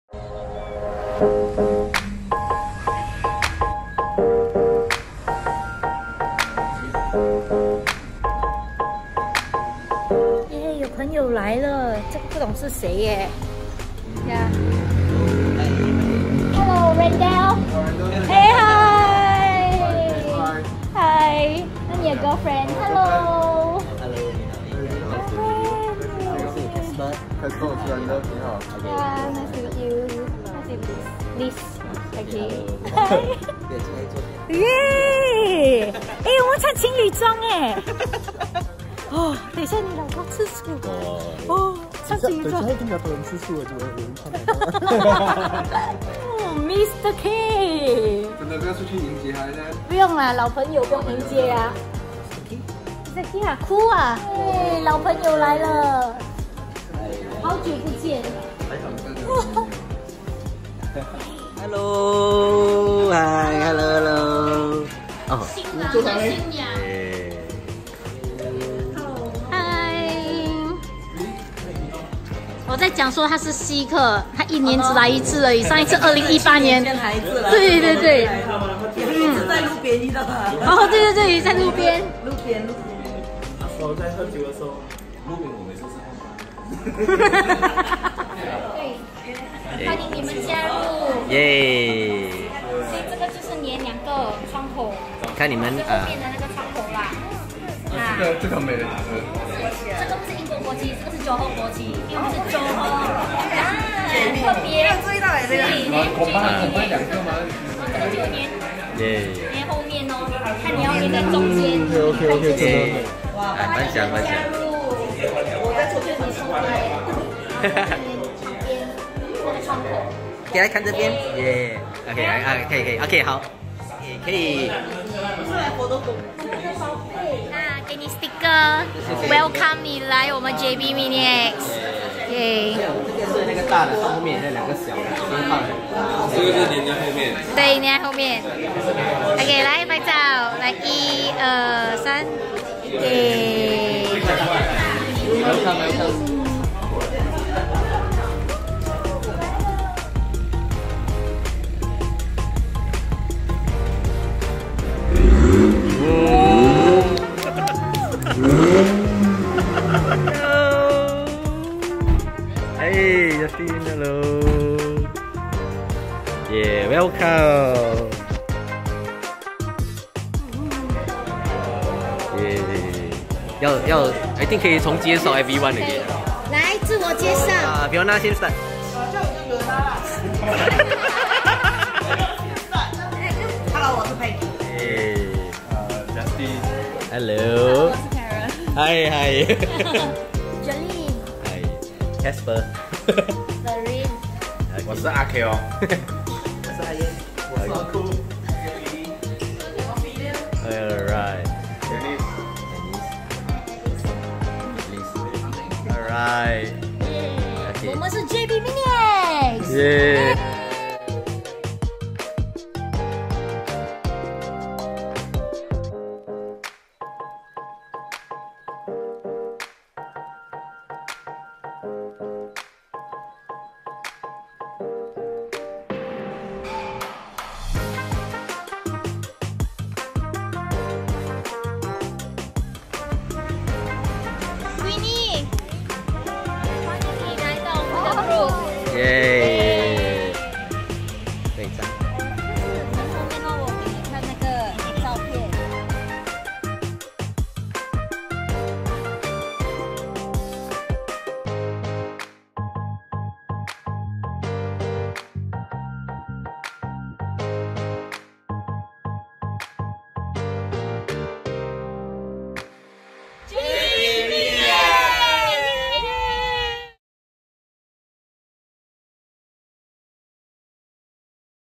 JB MINIacs x I Drink Coffee Vlog #05 [Welcoming 9 New Members!] 20191018
We appreciate and thank all the old friends and new friends who made time & effort to join us this evening. It was a great catch-up with old friends and always awesome to find new MINIacs in JB to join us. I always enjoy how cozy our group is and everyone is like a family to each and everyone and we always have tons of endless topics to talk about.

Can't wait to meet you guys again! Till the next meet up!

If you are a JB MINI owners, feel free to join us! Visiting JB? Come look for us too! We will be glad to host you!

Filmed by: Gerlyn & Casper
Organised by: Gerlyn, Casper & Viona (Qianna)
Attended by: Ah Yan, Justin, Karen, Ah Meng, Ophelia, Peggy, Willy, Koo, Ah Keong, Faeryn
New Members: Randall, Liz, Ivy, Dennis, Alice, Elvis, Louis, Li Qin & Queeny.

Location: G-14 & G-16, Jalan Ponderosa 2/1, Taman Ponderosa, 81100 Johor Bahru, Johor, Malaysia